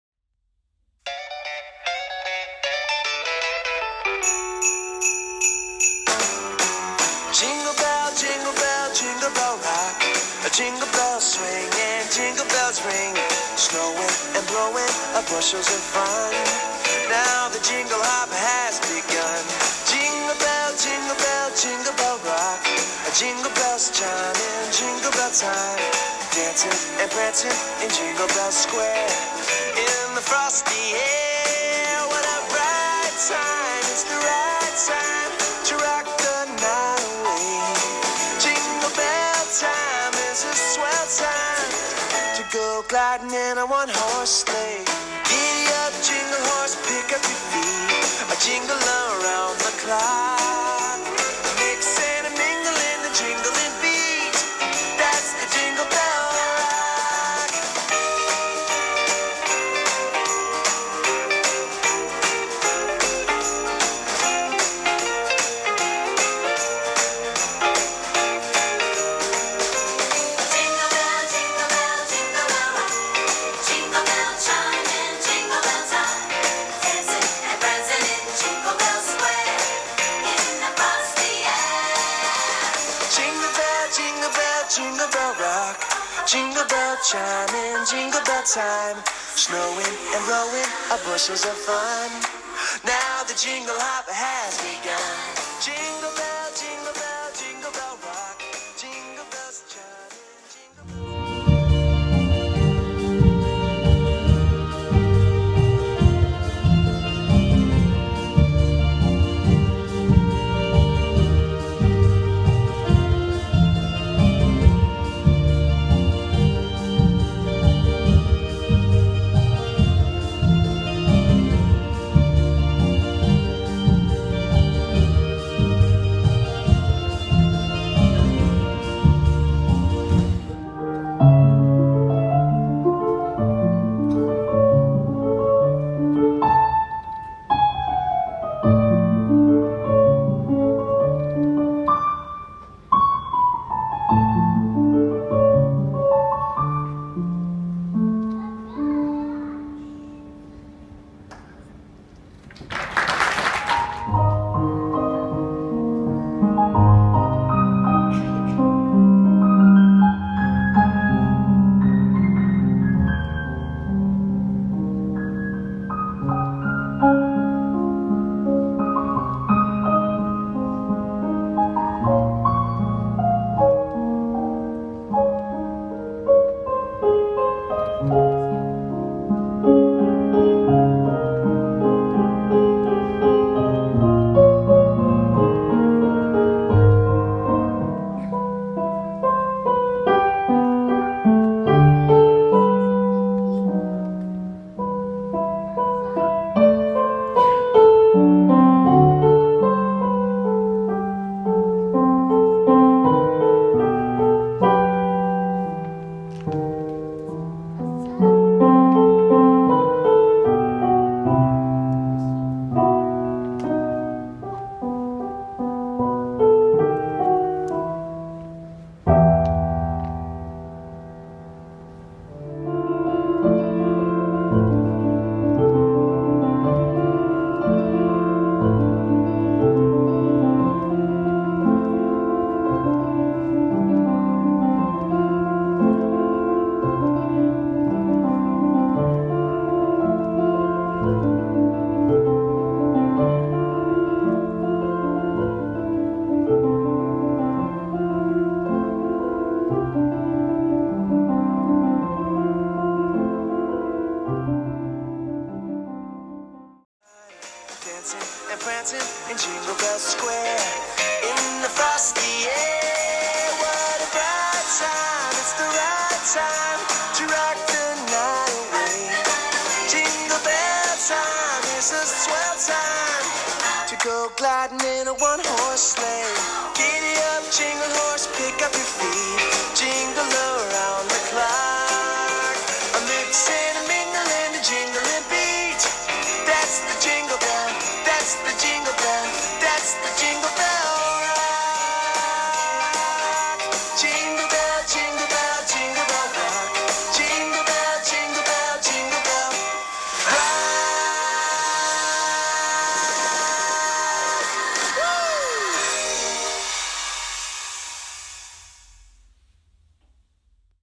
Concert de Noël de l'école de musique de Marmoutier
Souvenirs du concert "Noël en fête" qui a eu lieu le 18 décembre 2022 à la salle communale de Marmoutier.